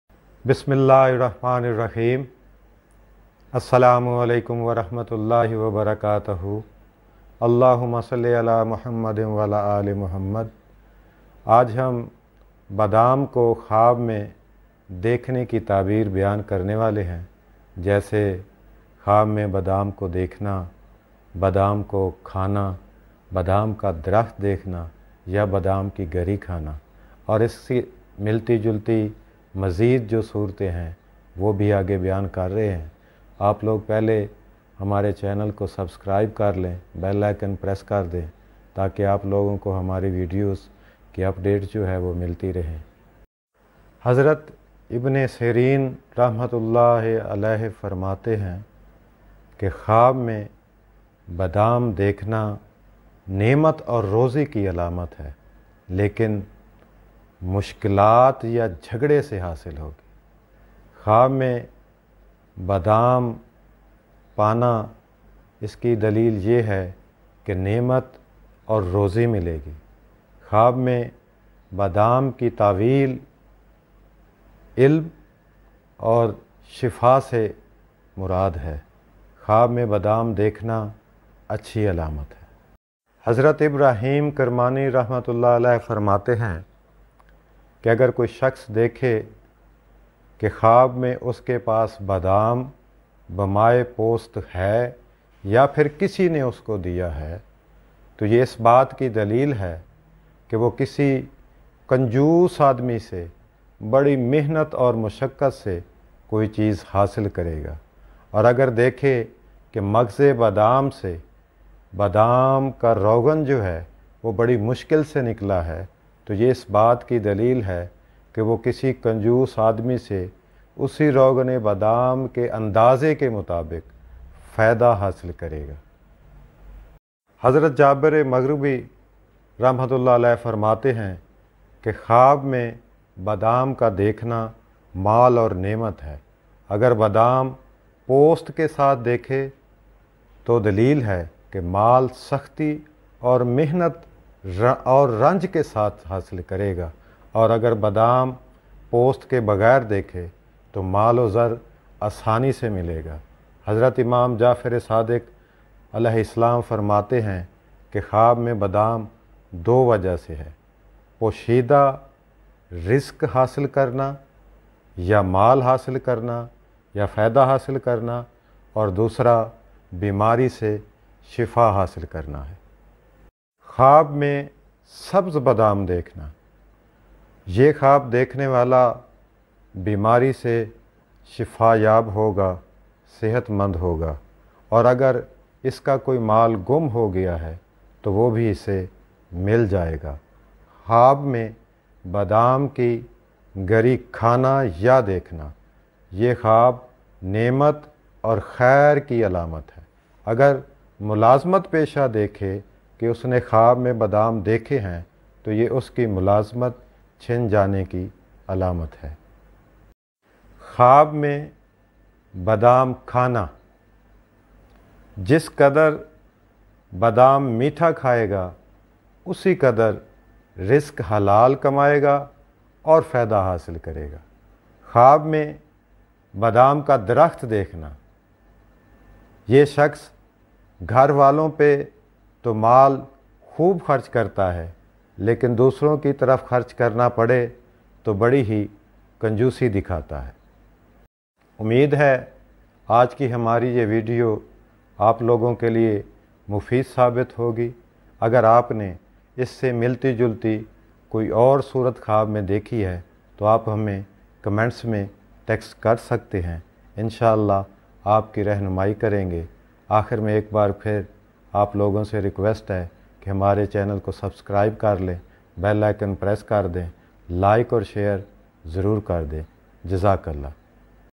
Khwab Mein Badam Dekhna || Badam Khana || Badam Lena || Badam Ka Darkht Dekhne Ki Tabeer
Welcome to My Youtube Dream Describing Channel "Mian Ghazzaal"

In the interpretation of the dream, we are describing Khwab Mein Badam Dekhna, Badam Khana, Badam Lena, Badam Ka Darkht Dekhne Ki Tabeer,
Badam Ki Garri Khana, Badam Sabz Rang Ka Dekhna, Badam Post Ky Sath Dekhna,

Please Like & Shar The Videos.
Thanks